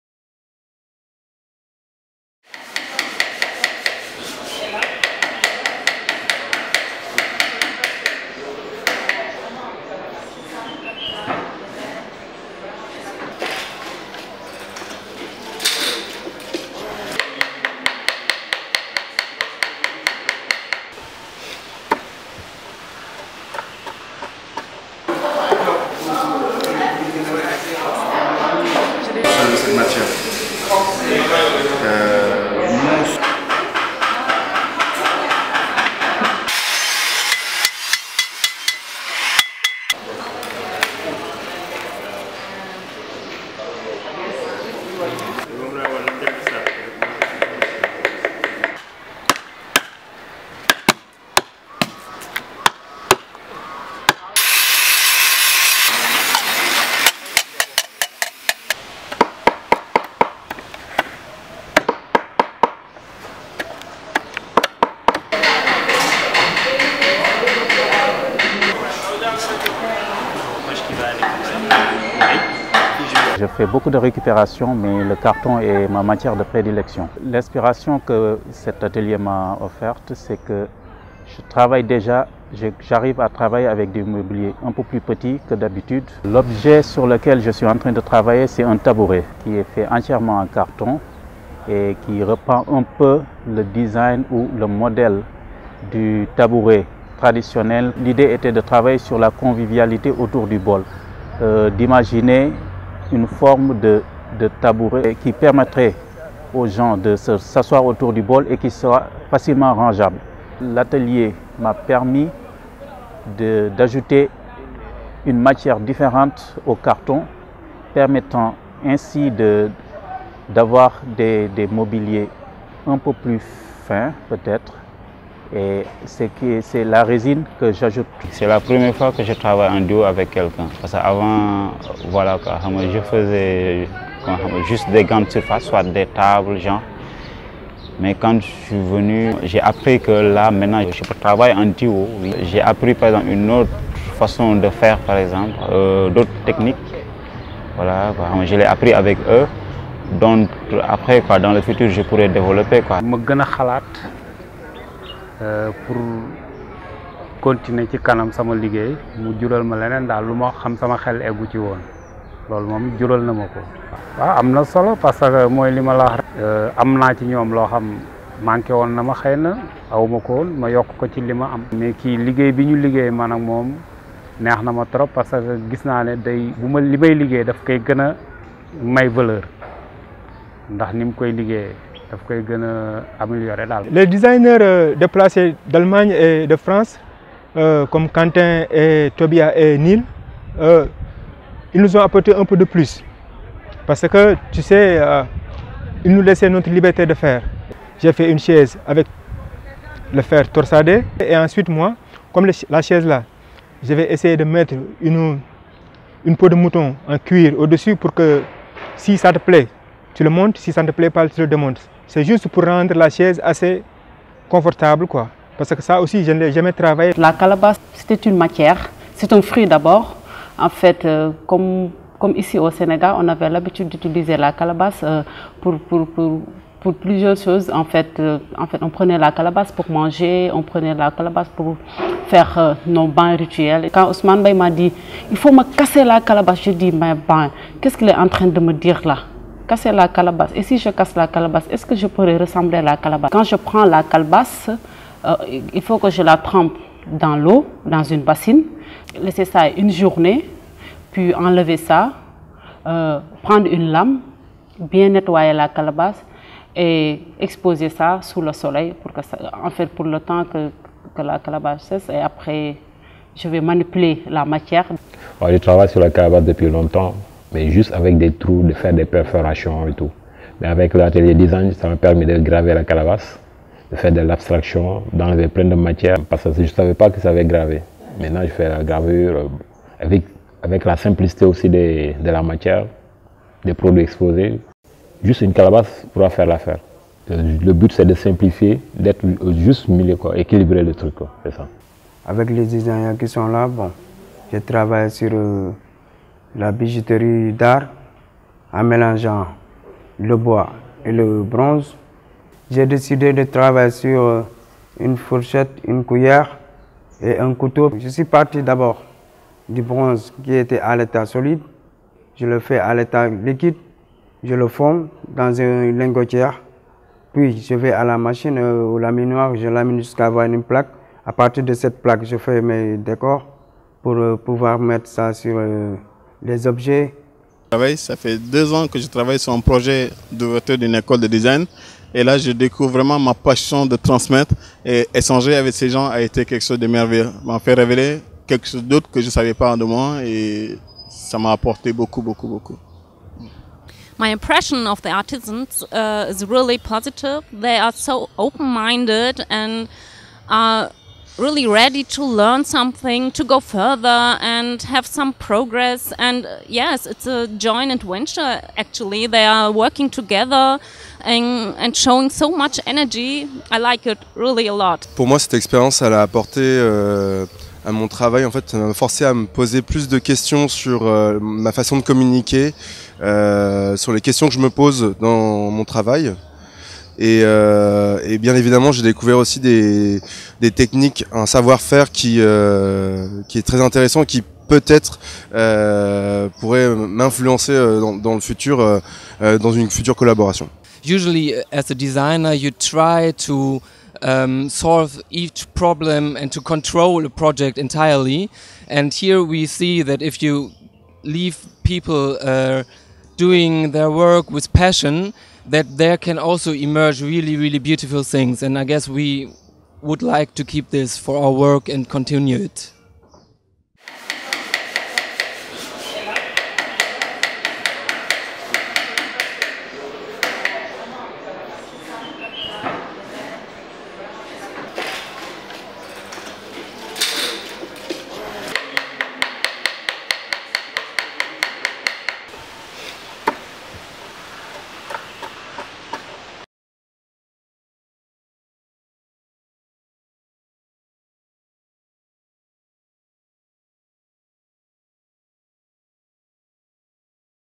Atelier Keur Design
De nombreux objets issus du savoir-faire traditionnel disparaissaient peu à peu des maisons sénégalaises.
Le projet KeurDesign vise à réintroduire dans les intérieurs sénégalais des objets du quotidien « made in Sénégal » mais aussi à accompagner la professionnalisation du secteur du design au Sénégal.
Du 23 avril au 5 mai 2018, des artisans sénégalais ont travaillé avec des designer français, allemands et sénégalais au Centre de Formation Artisanale (CEFA) afin de revisiter des objets et ustensiles du quotidien.

KeurDesign est un projet de l’Institut Français et du Goethe-Institut,  en collaboration avec le Centre de Formation Artisanale De Dakar et subventionné par le Fonds culturel franco-allemand.

Designers : Ousmane Mbaye (Sénégal) , Nil Atalay et Tobias Juretzek (Studio Nito / Allemagne) et Quentin Vuong (France)

Coordination du projet : Maya Ayad Mroueh

Participants :  Papis DIOP (Plasticien / Designer), Abdoulaye MBAYE (Peintre), Papa Mamadou NDOYE (Menuisier / Sculpteur), Samba SOW (Designer / Sculpteur), Demba Tidiagne GADIAGA (Sculpteur), Martin BASSENE (Sculpteur), Serigne GUEYE (Ferronnier), Mohamed ABBAS (Architecte), Ramatoulaye DANFA (Styliste), Lucien SAMBOU (Bijoutier), Yaye Fatou MANÉ NDIAYE (Styliste), Ass Malick BOPP (Sculpteur sur bois), Papa Mamadou NDOYE (Menuisier / Graveur), El Hadj Abdoulaye DIALLO (Cordonnier / Maroquinier) et Anna Gueye (Chercheuse).

Réalisation : Stéphanie Nikolaïdis
Image : Ousmane Lo
Montage : Stéphanie Nikolaïdis et Ousmane Lo

Avril - Mai 2018